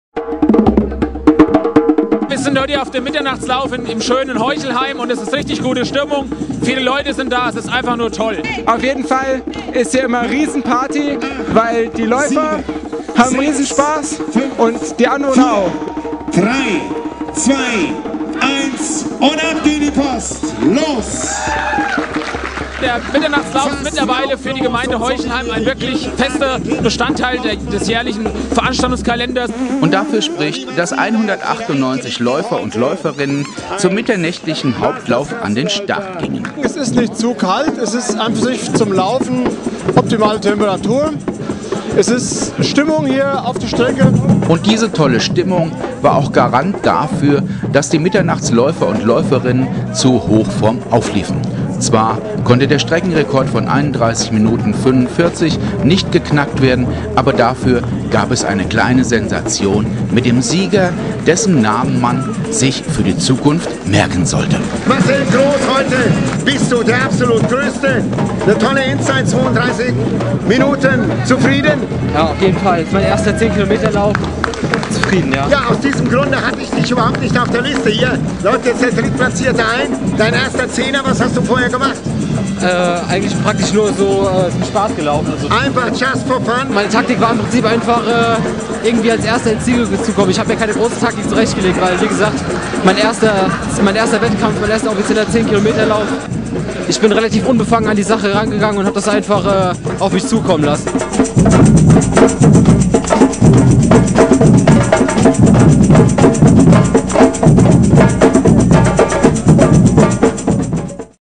Mitternachtslauf in Heuchelheim
16. Heuchelheimer Mitternachtslauf: Samba-Klänge, angenehme kühle Lauftemperaturen, Party entlang der Strecke und dann auch noch ein Newcomer als Sieger: Marcel Groß (25) vom TSV Krofdorf-Gleiberg in 32:11 min.